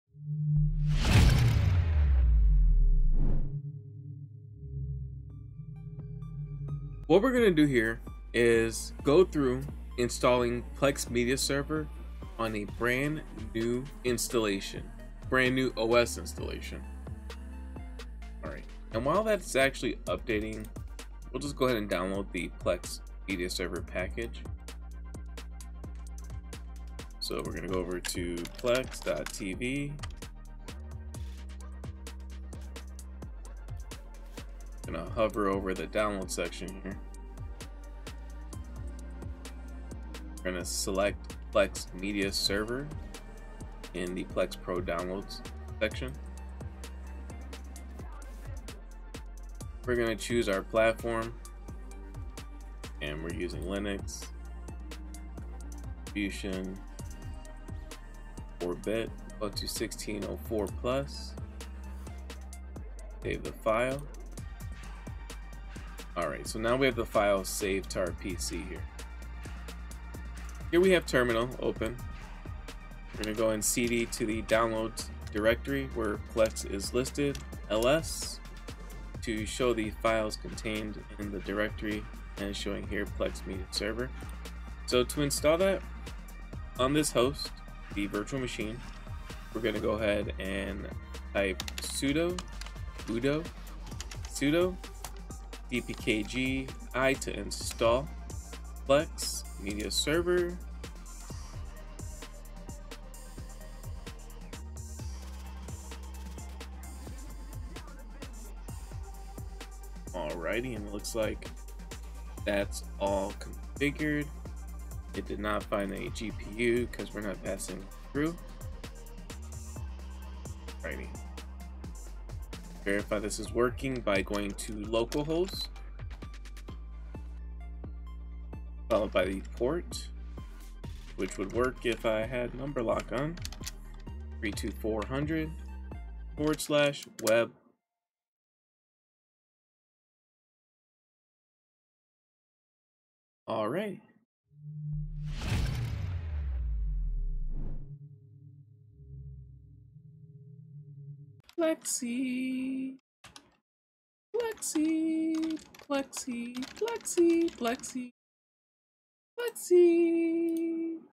PLEX Media Server live Setup 2022, on Ubuntu
PLEX Media Server complete install to Setup on Ubuntu 20.04 (VM) for BEGINNERS up to Setup wizard.